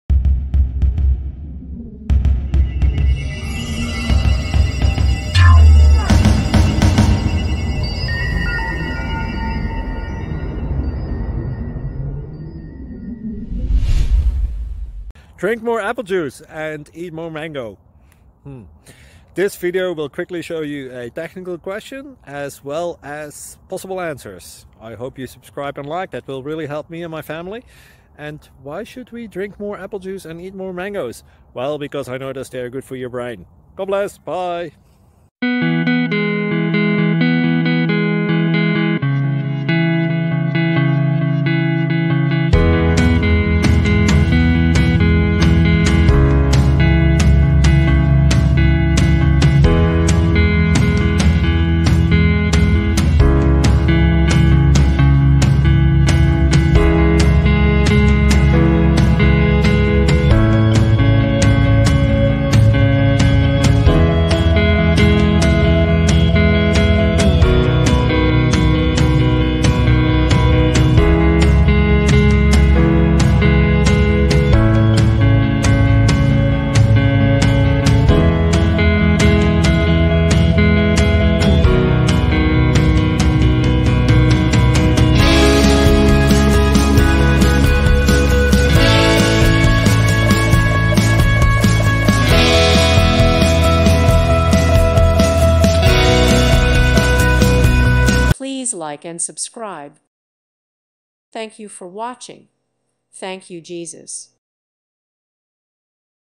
Which layers should reflect the domain language (if a domain language can strictly exist)?
Software Engineering: Which layers should reflect the domain language (if a domain language can strictly exist)?

With thanks & praise to God, and with thanks to the many people who have made this project possible! You are responsible for your own actions.